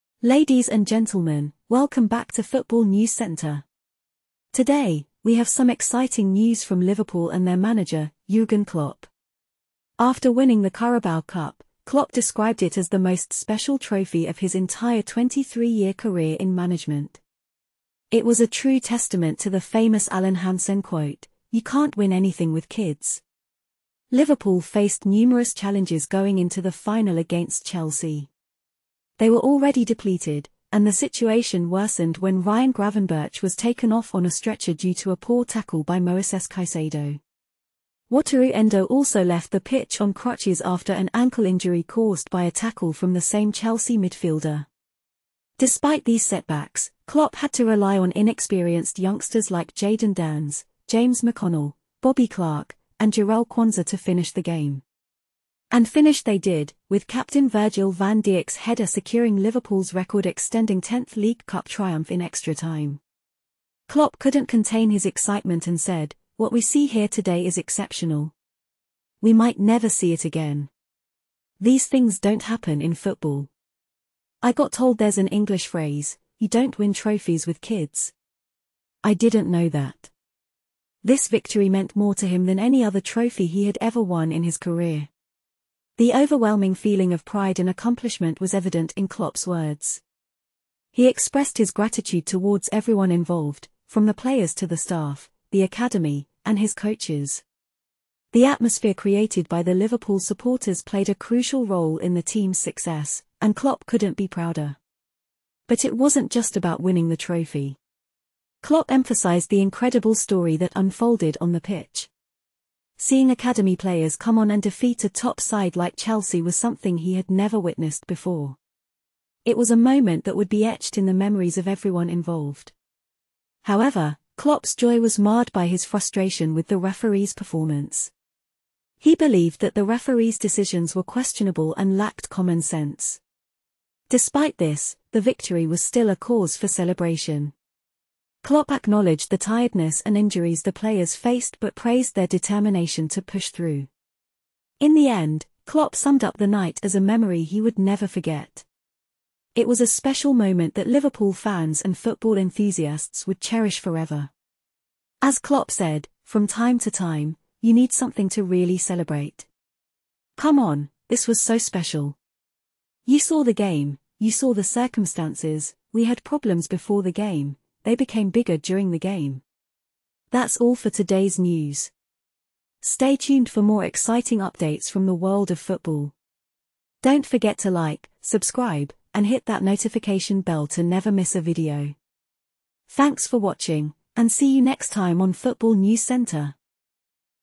Jürgen Klopp hails Liverpool triumph as ‘most special, a memory for ever’
Jürgen Klopp, the manager of Liverpool, has described their Carabao Cup triumph as the most special trophy of his 23-year managerial career. Liverpool won the final against Chelsea despite being depleted due to injuries and finishing the match with inexperienced youngsters on the pitch. Klopp praised the team's performance and the contribution of everyone involved, including the academy players who came on and helped secure the victory. He stated that it was an overwhelming feeling and a memory that will never be forgotten. However, Klopp was critical of the referee's performance and appeared to ignore his offer of a handshake after the game. He expressed frustration with the referee's decisions and lack of common sense. Despite the win, Klopp emphasized that he is not concerned about his legacy as a manager, but rather the loyalty and support of the club's fans. Klopp concluded by saying that the victory was a special moment of celebration and a cherished memory.